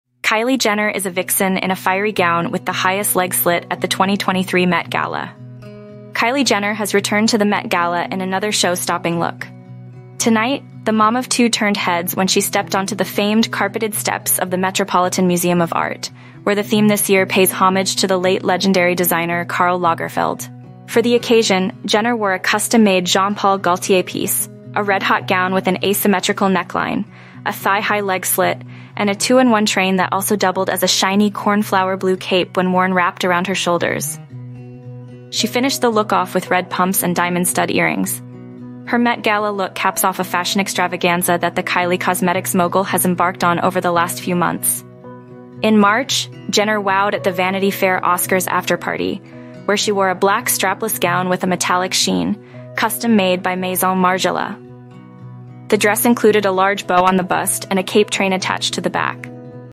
Kylie Jenner Is a Vixen in a Fiery Gown with the Highest Leg Slit at the 2023 Met Gala
Kylie Jenner has returned to the Met Gala in another show stopping look.

Tonight, the mom-of-two turned heads when she stepped onto the famed carpeted steps of the Metropolitan Museum of Art, where the theme this year pays homage to the late legendary designer Karl Lagerfeld.

For the occasion, Jenner wore a custom-made Jean Paul Gaultier piece: a red-hot gown with an asymmetrical neckline, a thigh-high leg slit, and a two-in-one train that also doubled as a shiny cornflower blue cape when worn wrapped around her shoulders. She finished the look off with red pumps and diamond stud earrings.